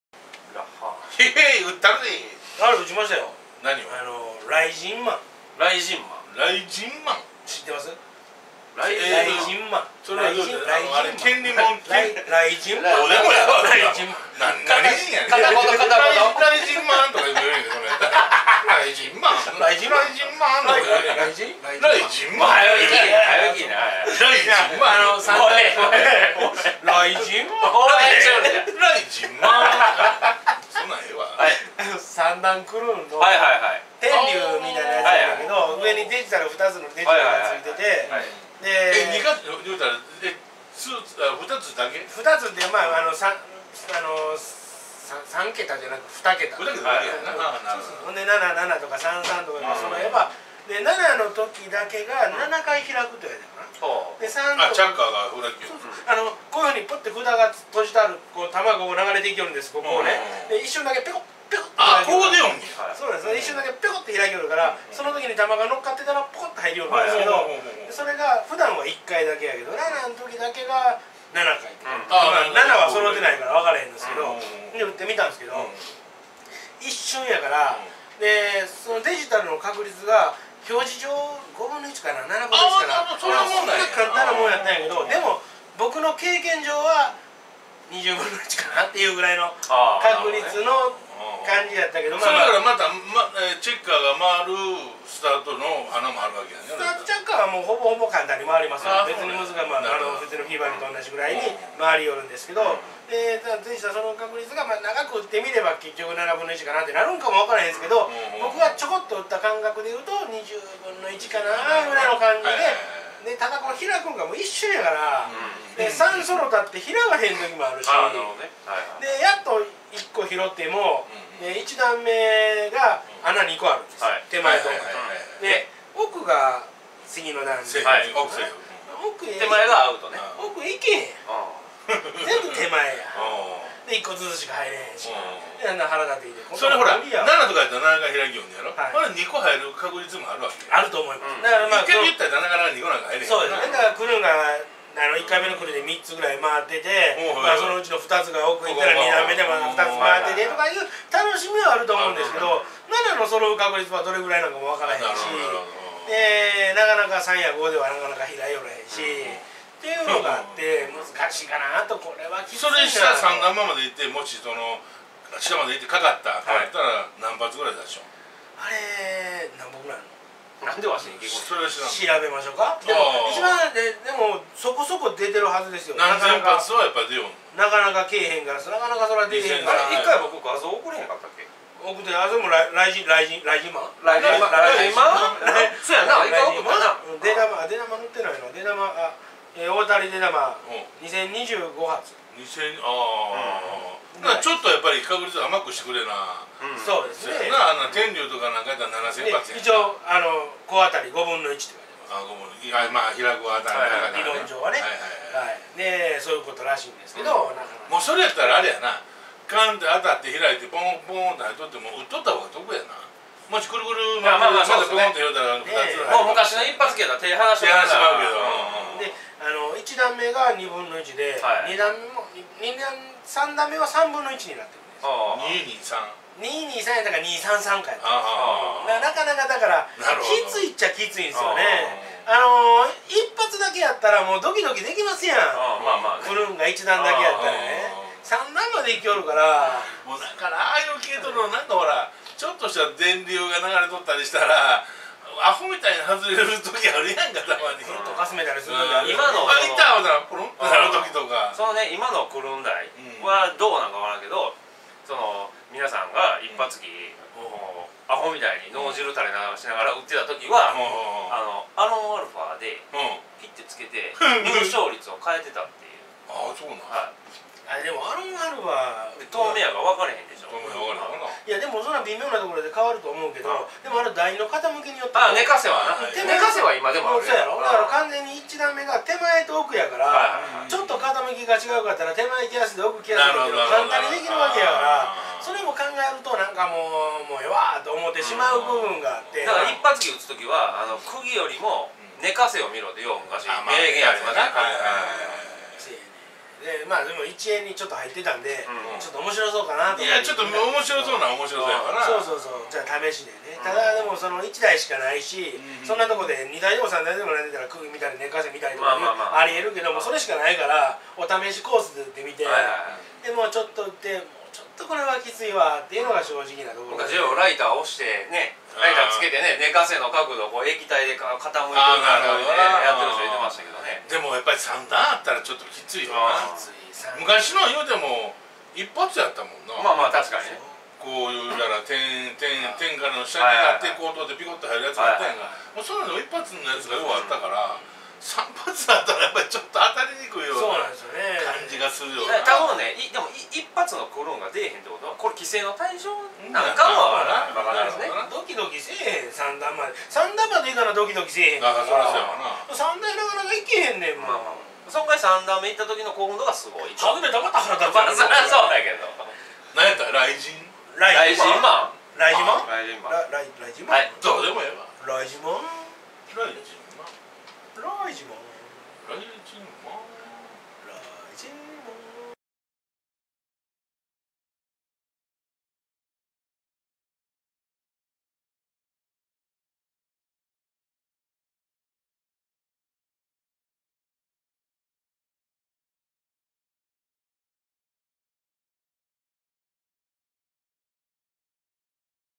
パチンコ台の傾き出玉が変わる!!! ドキドキ出来ない!!! #528
大阪で活動する芸人を中心にお気楽に面白トークをしていきます。 芸能界の裏話やパチンコの攻略法も聞けるかも！！！

出演者
若井りき
黒井博之
マタキタマタコイヤ

撮影協力
大阪ミナミのB168プロレス食堂『ちゃん唐』